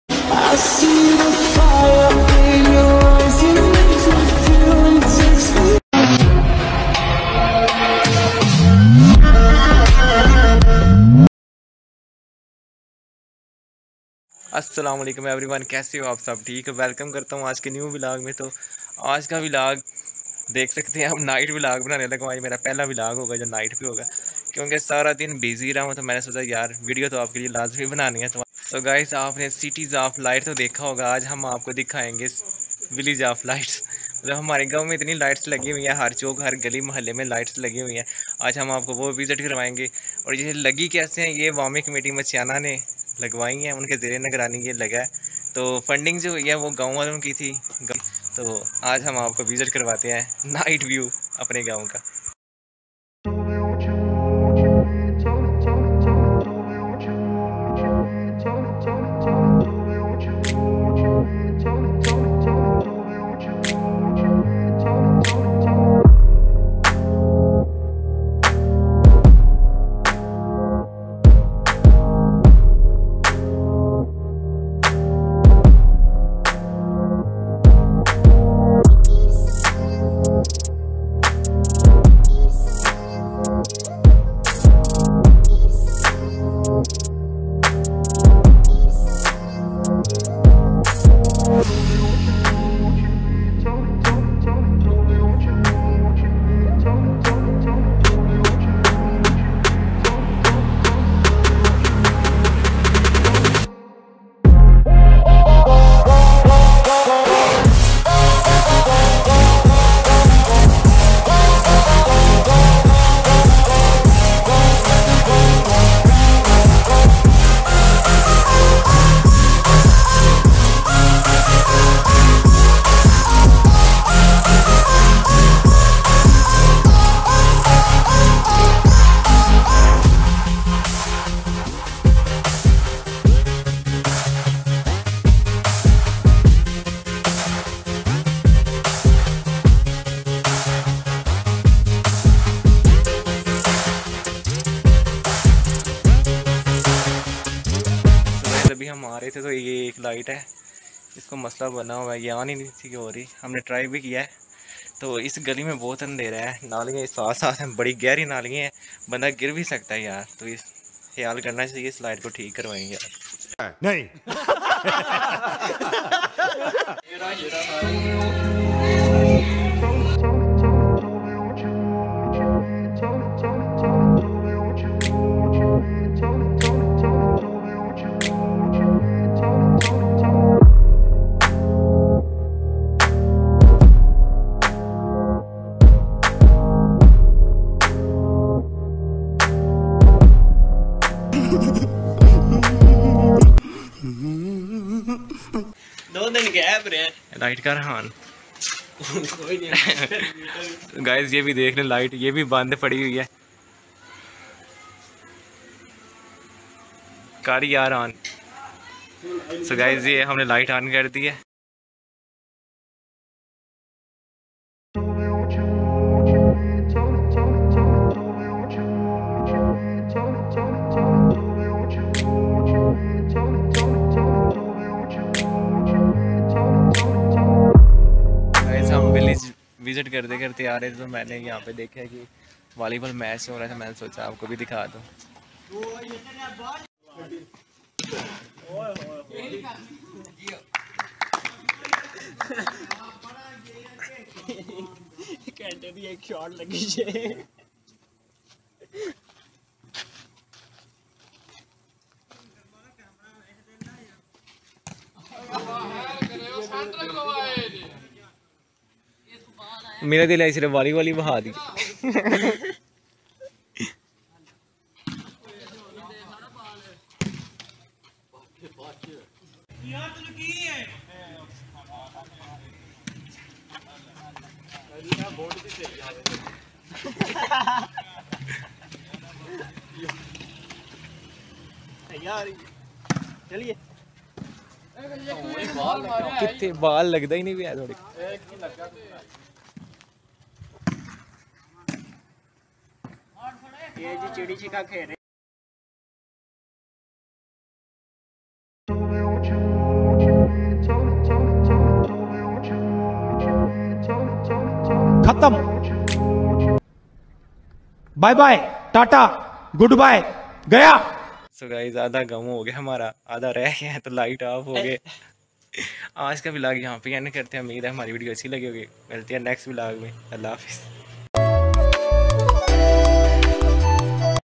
My First Night VLog To Visit Lights Of Village
welcome To Village VLogs 🤗♥️ !

 Today i Share With You My first Night vLog To visit Village kf Light Machhiana City Gujrat  🇵🇰😗

Here is My New vLog Video of My channel 😍 Hey guys! Today I'll be share in this video I will be showing you Full Visit my village at Night Lights projects